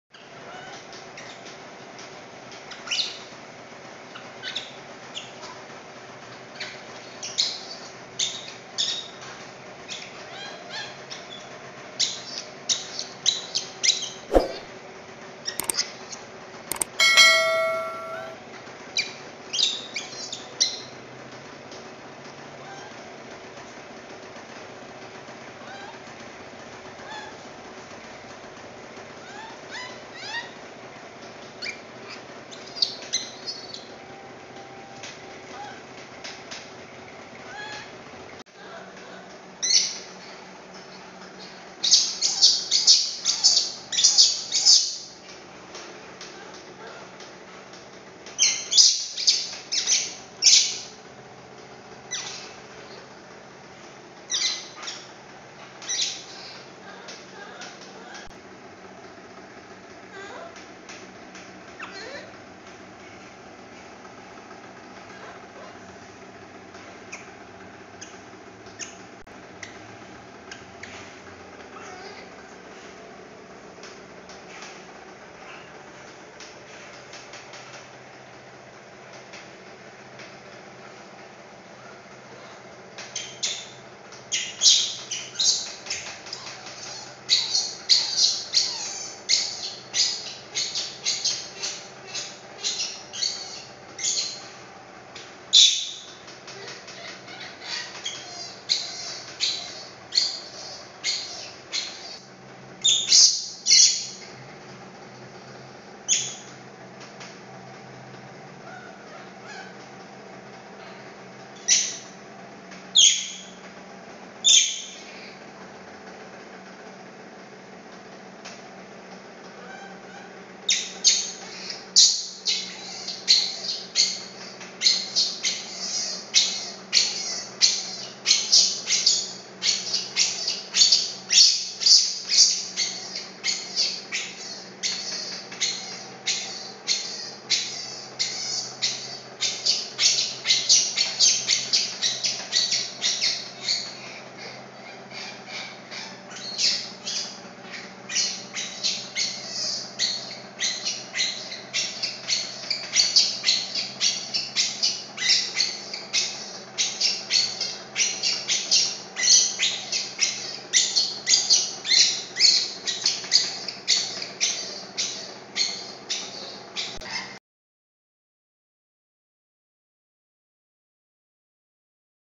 BABY MONKEY CRY MISSING
I PRAY THA MY PLAN MAKES A SURPRISE, MY FRIEND SAYS... THE BABY MONKEY SEE ME FROM THE WINDOW, SO THE BABY Screamed at the WINDOW...T YOU WILL BE GIVEN A LONG LIFE AND ABUNDANT FORTUNE.